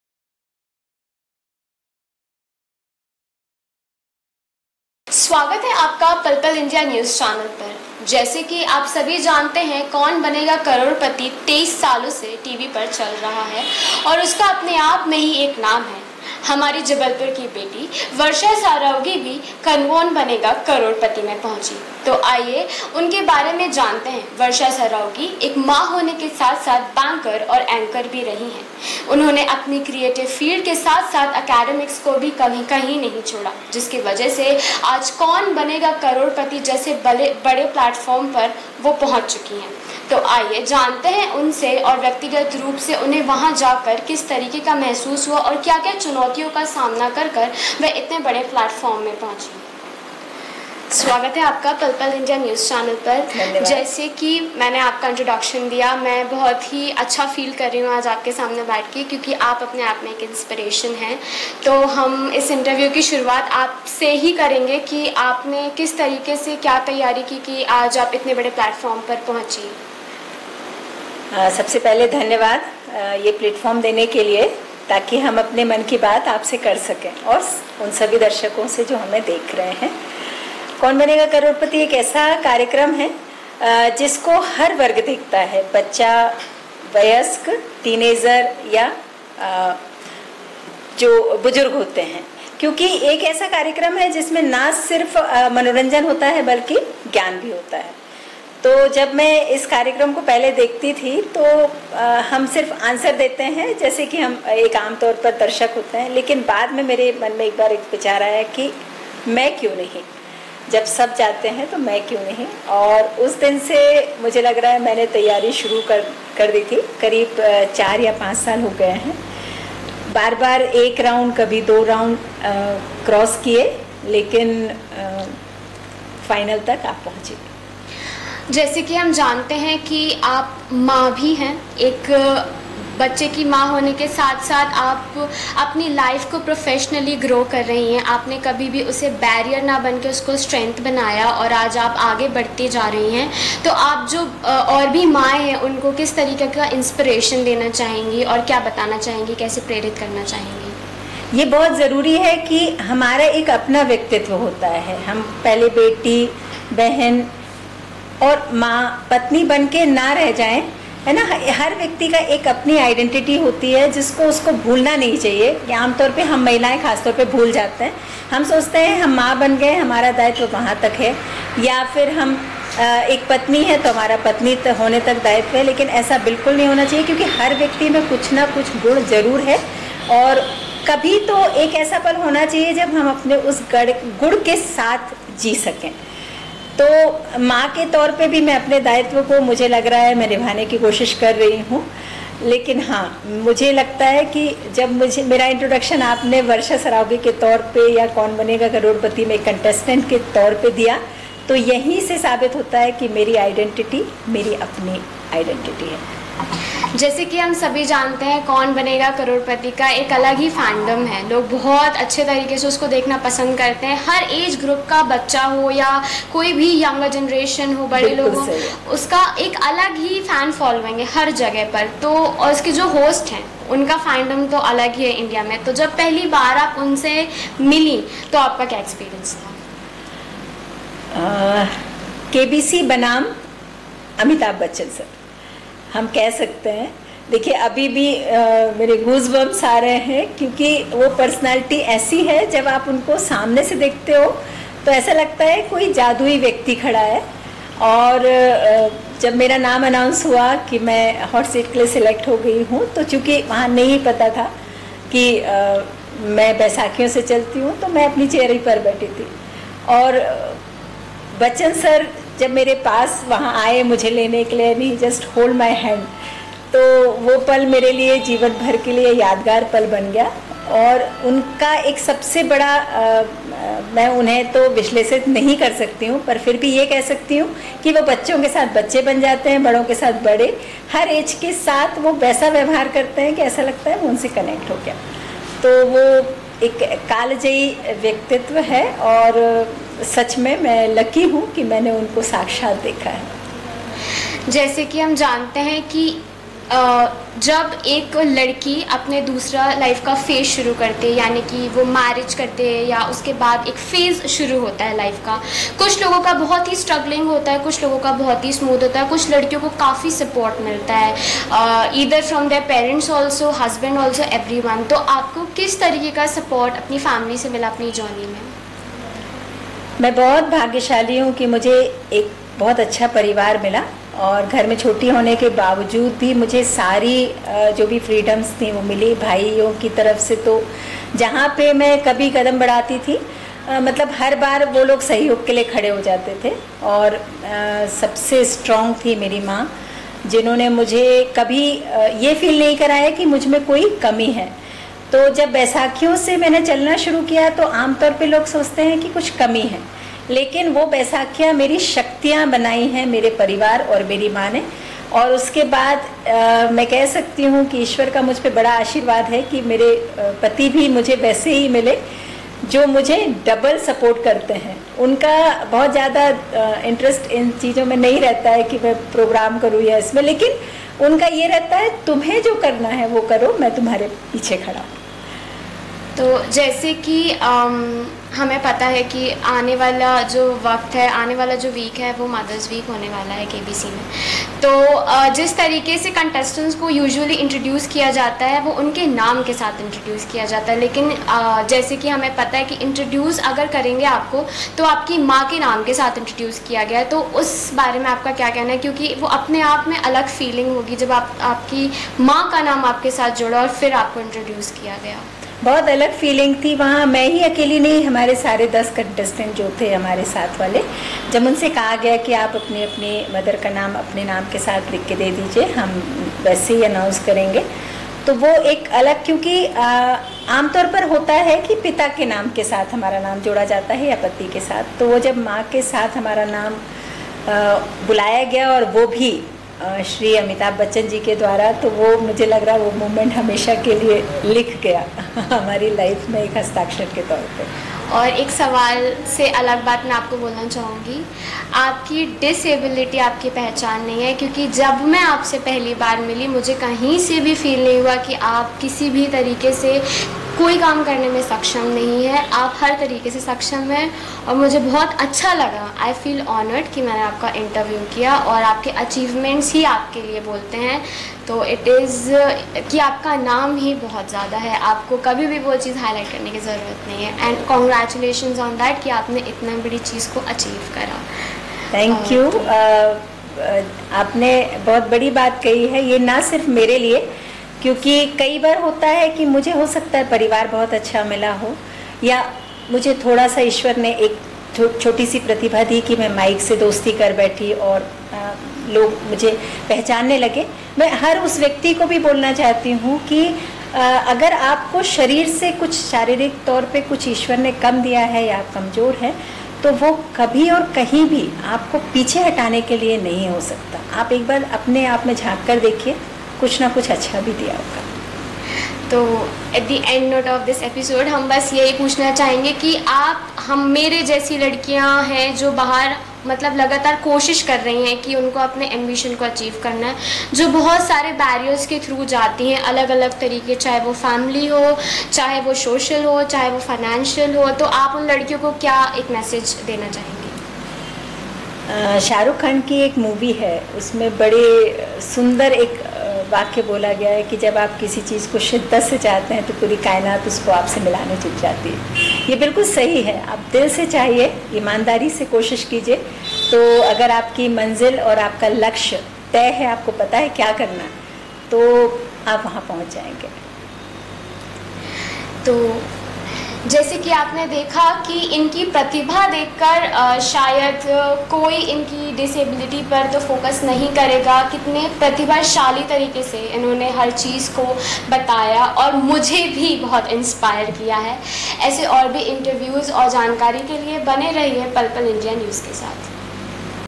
केबीसी के हॉट सीट तक पहुंची जबलपुर की वर्षा सरावगी | KBC Versha Saravgi Jabalpur
जबलपुर - केबीसी में हॉट सीट तक पहुंची जबलपुर की वर्षा सरावगी
केबीसी में पहुंचकर बढ़ाया संस्कारधानी का मान
एक मां होने के साथ-साथ प्रोफेशनल बैंकर भी हैं वर्षा
केबीसी में जाने के लिए 2 साल से कर रहीं थीं तैयारी
दृढ़ निश्चय ने पहुंचाया केबीसी के मंच तक
दिव्यांग होने के बावजूद कड़ी मेहनत से पहुंची केबीसी में